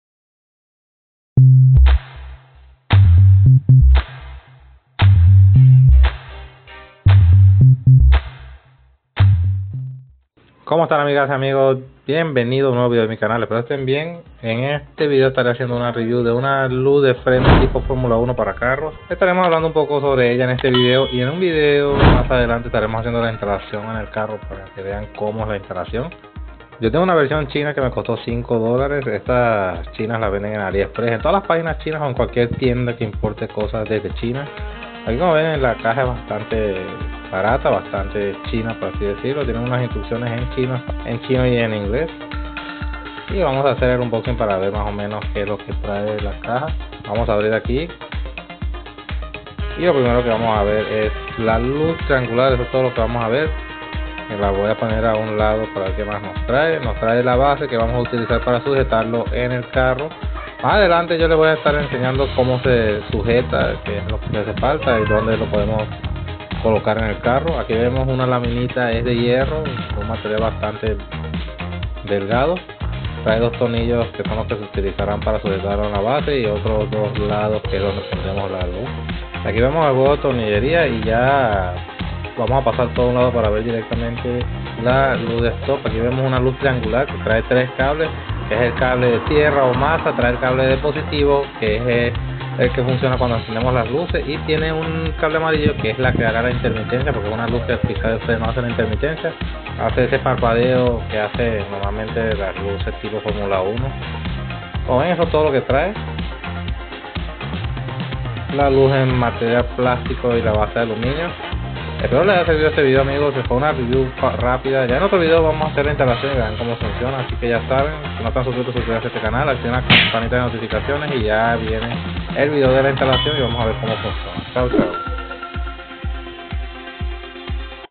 luz de freno tipo formula 1
Que tal amigos en este video voy a estar haciendo una reseña de una luz de freno o stop triangular tipo formula 1, estaré hablando de sus materiales y veremos si vale la pena o no comprar esta luz de freno.